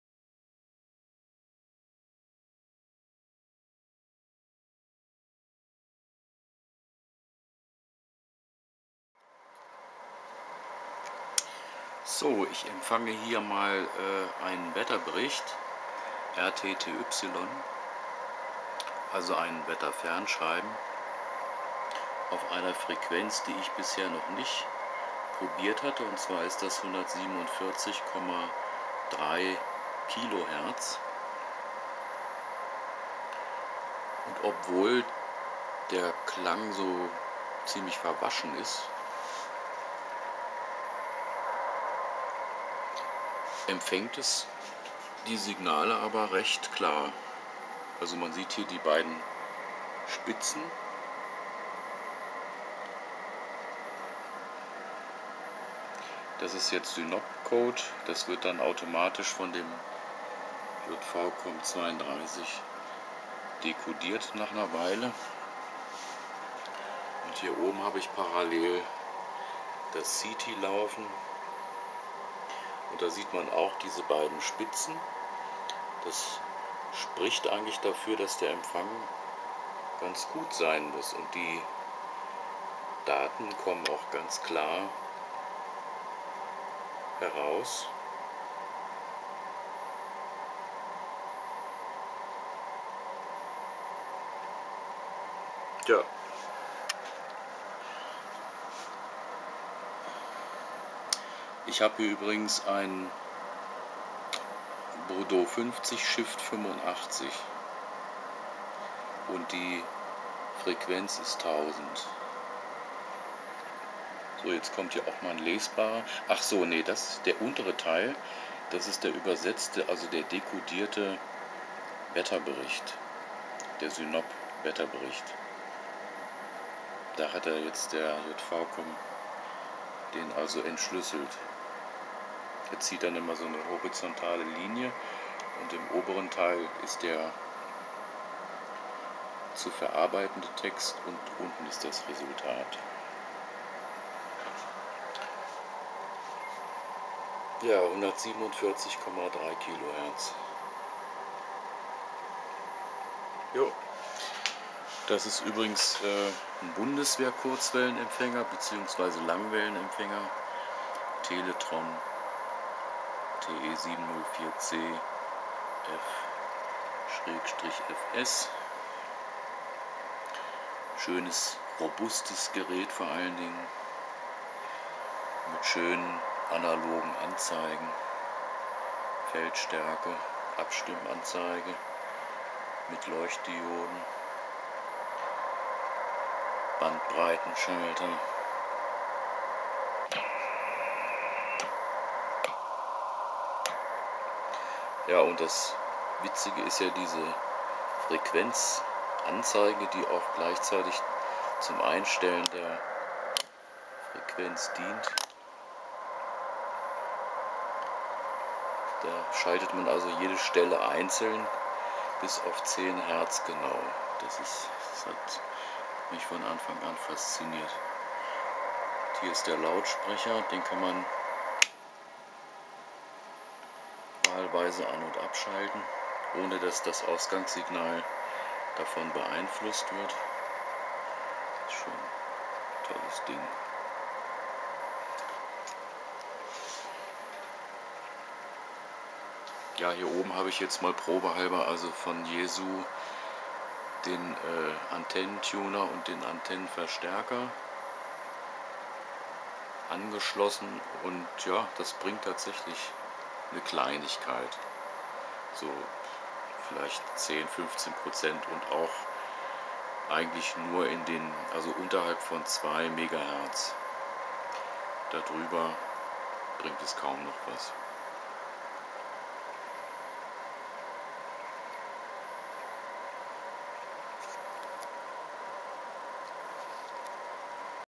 DWD 147,3 khz Wetterbericht RTTY Teletron TE 704 C - F/FS
Bundeswehr Empfänger für Kurzwelle, Mittelwelle und Langwelle "Teletron TE 704 C - F/FS". Empfang eines RTTY Wetterberichts des DWD (Deutscher Wetterdienst) auf 147,3 khz (Langwelle). UTC 2:30, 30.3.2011
German Bundeswehr military ham radio "Teletron TE 704 C - F/FS" for short wave, medium wave and long wave. Reception of a RTTY weather report of the German weather forcast station DWD (Deutscher Wetterdienst) on 147,3 khz (long wave).